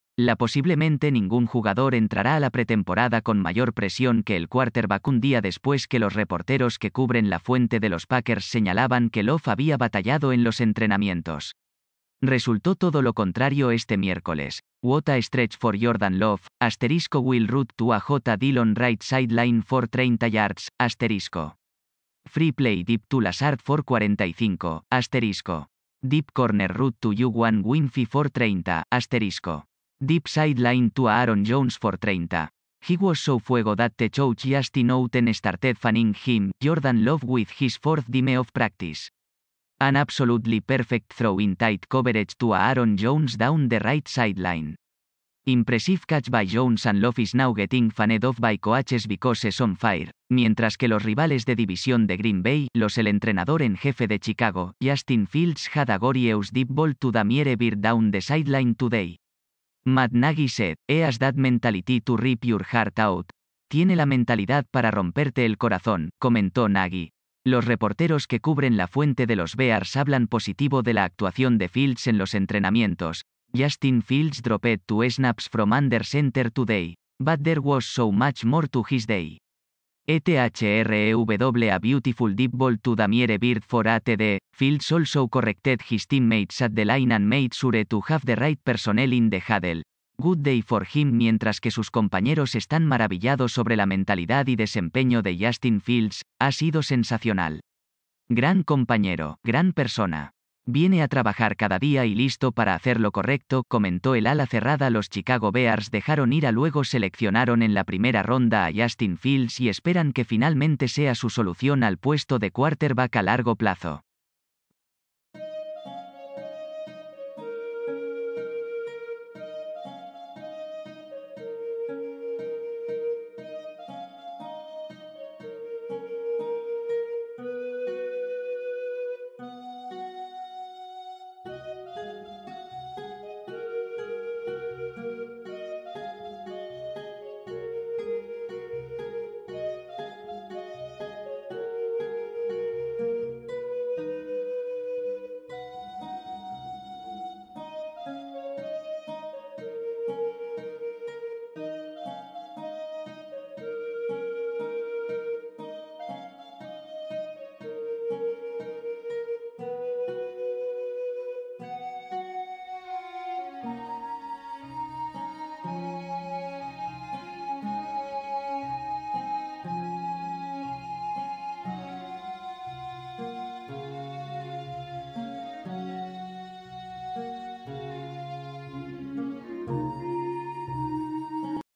Jordan Love y Justin Fields destacan en los entrenamientos de la NFL
LaPosiblemente ningún jugador entrará a la pretemporada con mayor presión que el quarterbackUn día después que los reporteros que cubren la fuente de los Packers señalaban que Love había batallado en los entrenamientos. Resultó todo lo contrario este miércoles.What a stretch for Jordan Love:* [...]